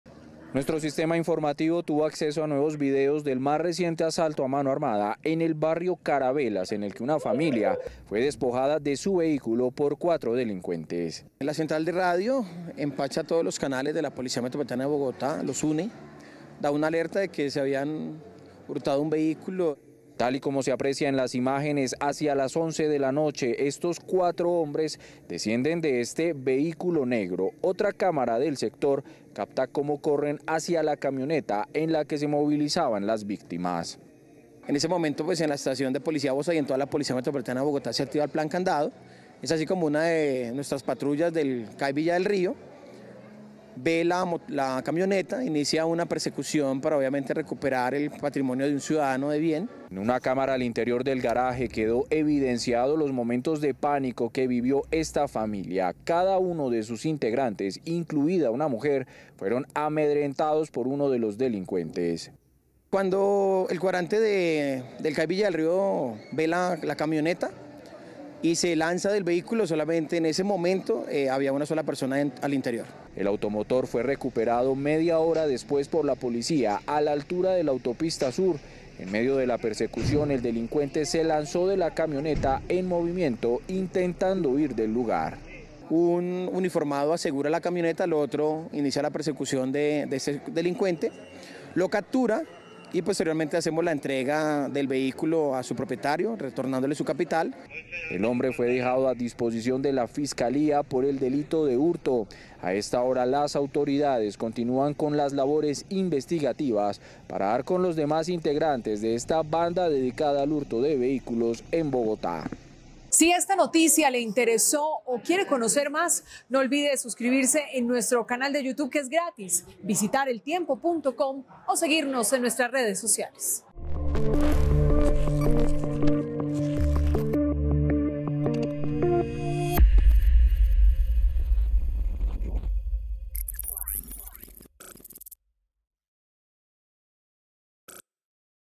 Fue capturado uno de cuatro delincuentes que protagonizaron un robo a mano armada | El Tiempo
Fue capturado uno de cuatro delincuentes que protagonizaron un robo a mano armada a una familia en Puente Aranda. El vehículo que fue robado fue encontrado la localidad de Bosa.